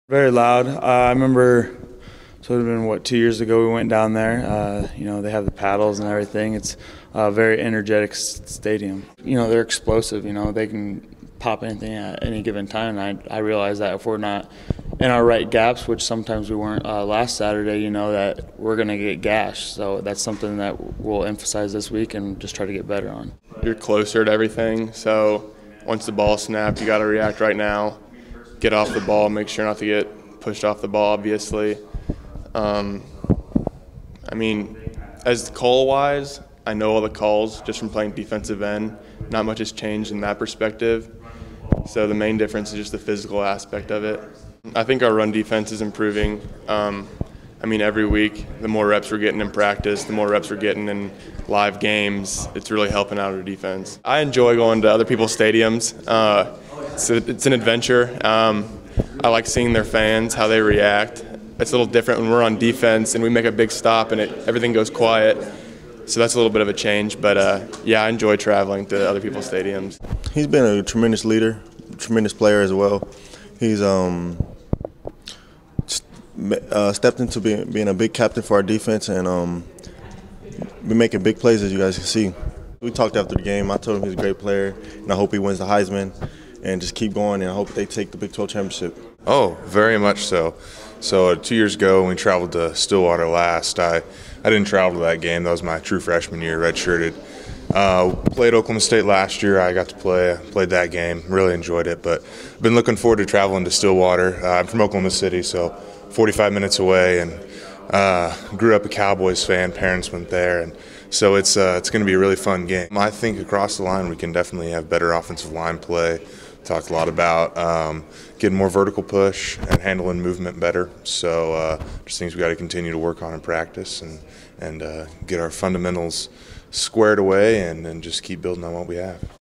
CFTV: Players take first look at Oklahoma State
Jevohn Miller, Mitchell Meyers, Kamari Cotton-Moya and Daniel Burton spoke with the media Monday. The players discussed the Oklahoma State Cowboys for the first time before their matchup at 11 am on Oct. 4 in Stillwater, Okla. Highlights in this CFTV.